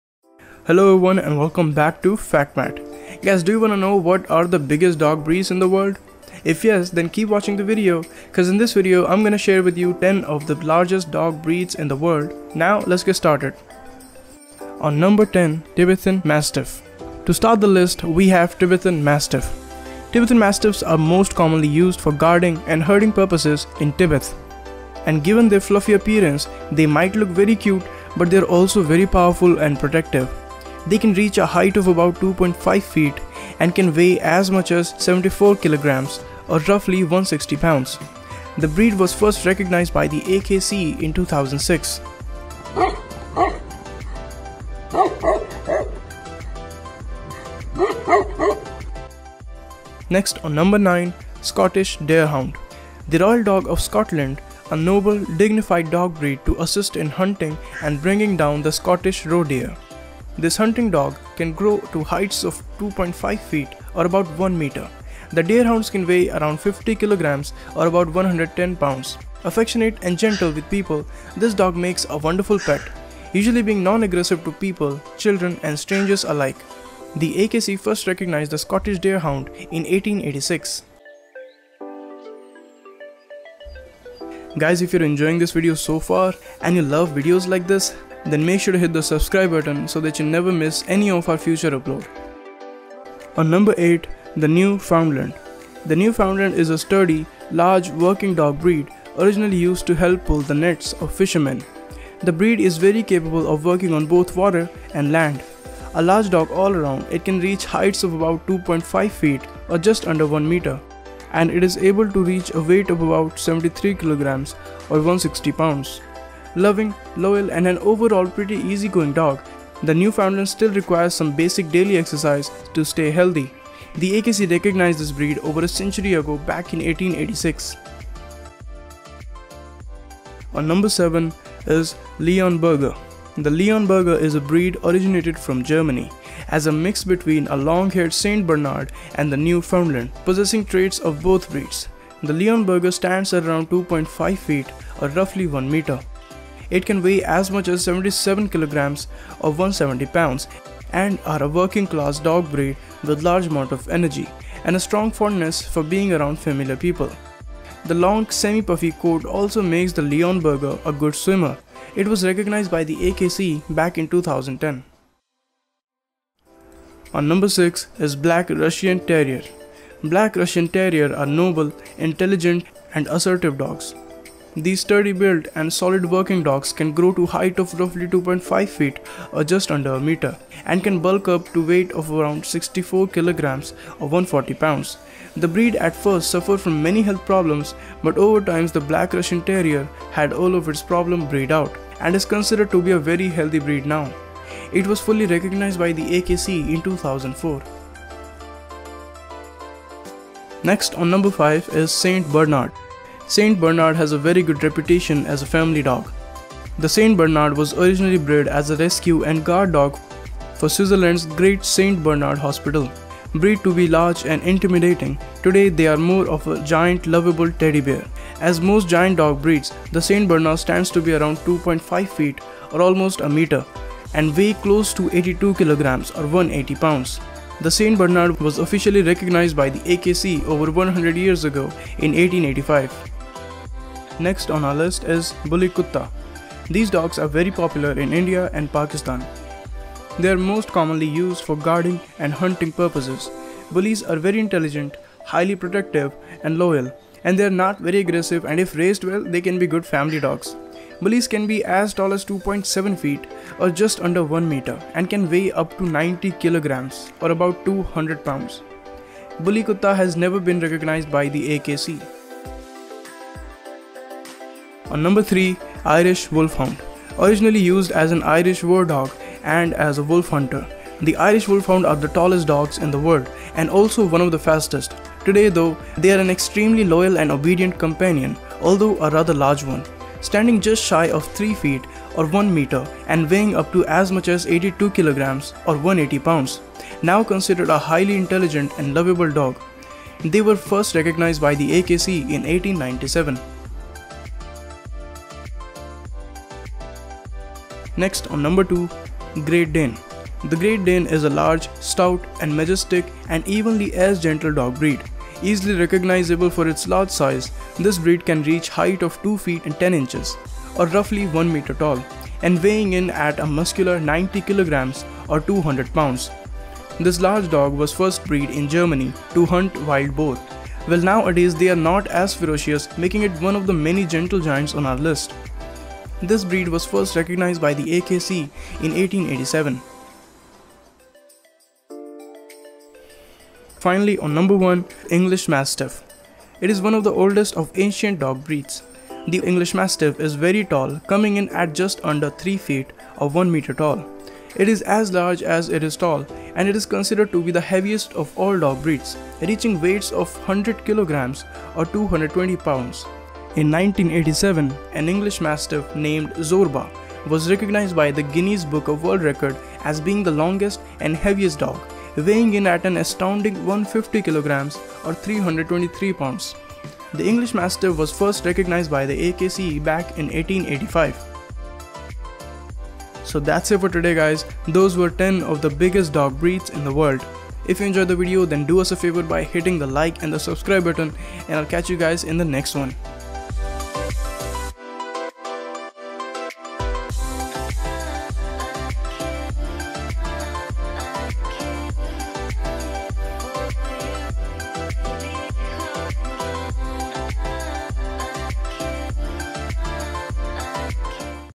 10 Largest Dog Breeds In the World | Biggest and Tallest Dogs
largest dog breeds in the world. Biggest dog breeds in the world.

Hi guys welcome back. In this video i have listed Top 10 biggest dog breeds in the world. List includes dogs like, Saint Bernard, bully kutta, Great Dane, Irish Wolfhound, etc
Our previous videos:
Timestamp
00:00 Intro
00:14 Dog 10
00:49 Dog 9
01:23 Like and subscribe
01:32 Dog 8
02:10 Dog 7
02:46 Dog 6
03:20 Dog 5
03:53 Dog 4
04:23 Dog 3
04:57 Dog 2
05:33 Dog 1
06:12 Outro